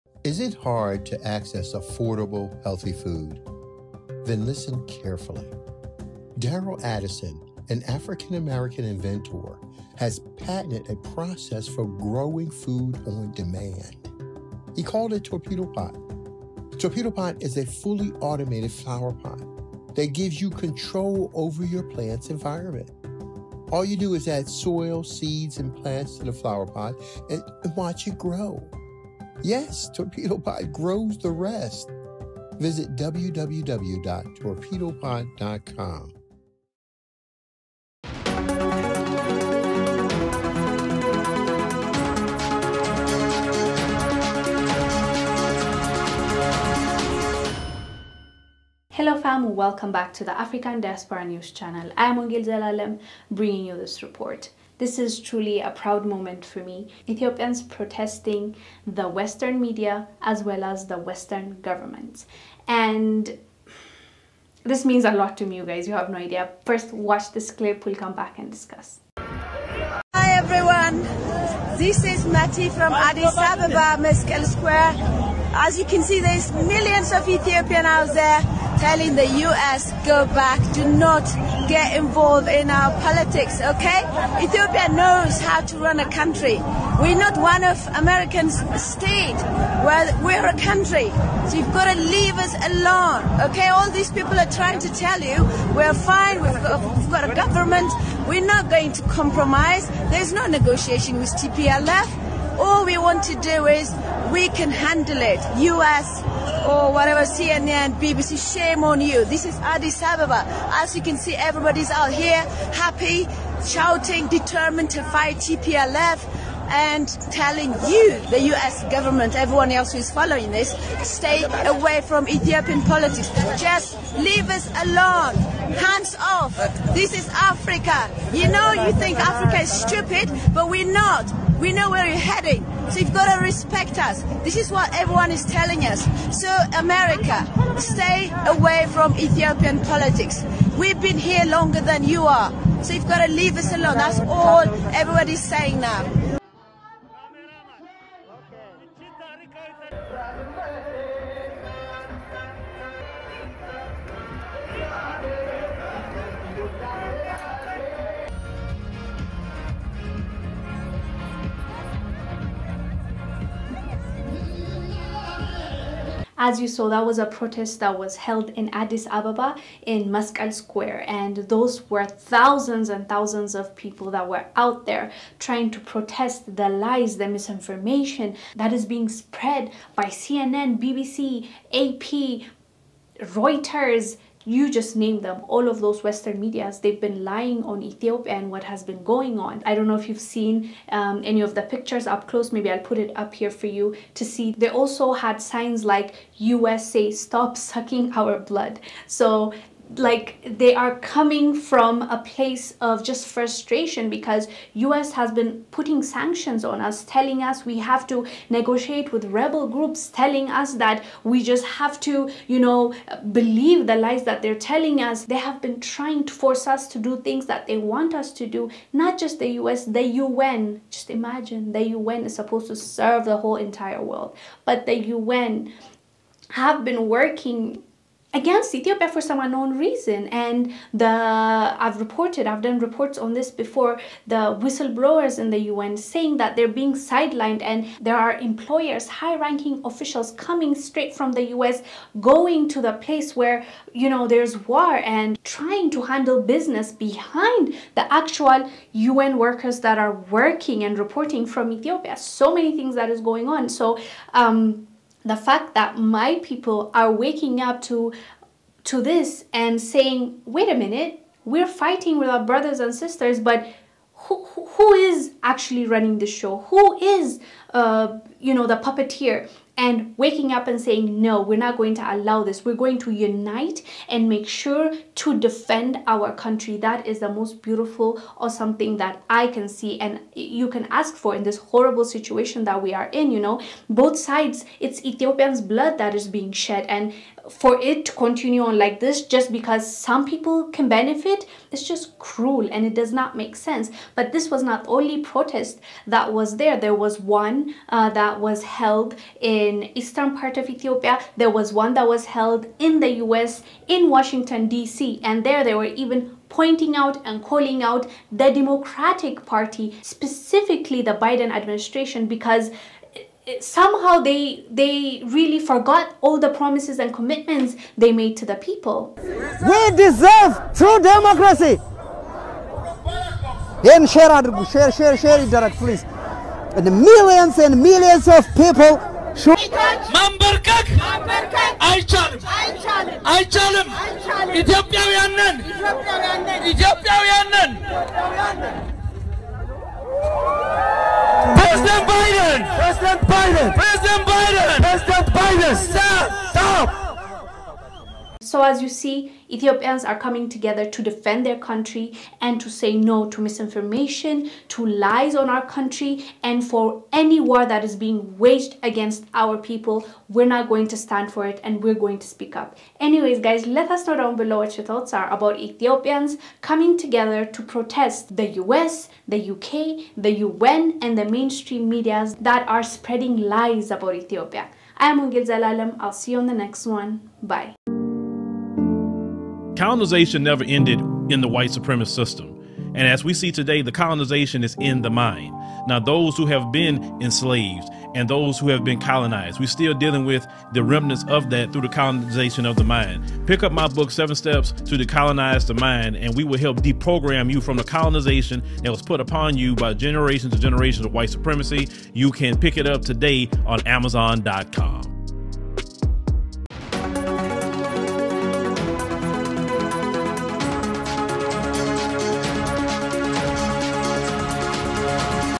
Ethiopians Protest The Lies That The Western Mainstream Media Is Pushing About Ethiopia
Wongel Zelalem reports on Ethiopians protesting the U.S. and western world mainstream media for the bias and inaccurate narrative they are spinning about Ethiopia.